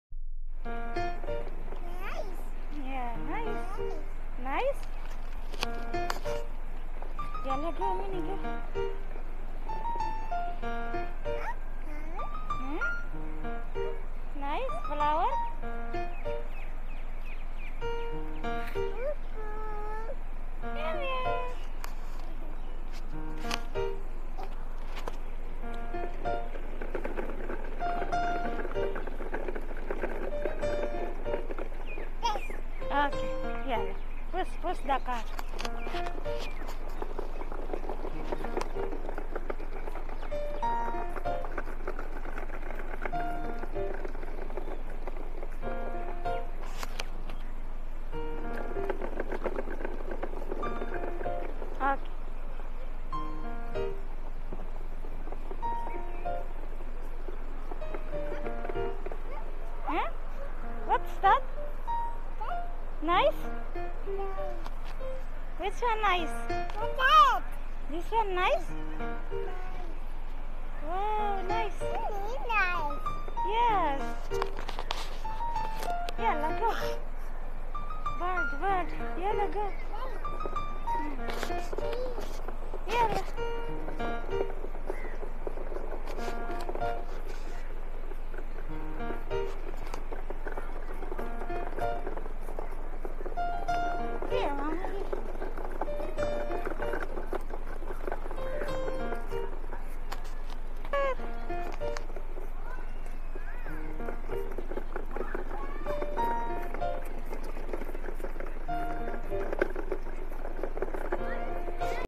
Walking 🚶‍♂️ in the park//Lailah Lovely Channel 😍
Walkedinthepark she's talk active baby Mashallah it was nice weather and nice view to walk 🚶‍♂️ 😍 lifeinUAE 🇦🇪. ♥